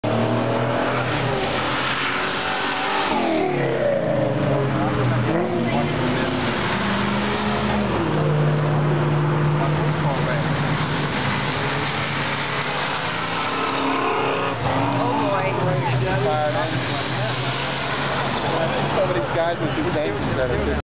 Mustang Week 2010 (4) Stangs Leaving Car Show (Gray n Burg Fox Body).AVI
Mustang Week 2010 Stangs Leaving Colonial Mall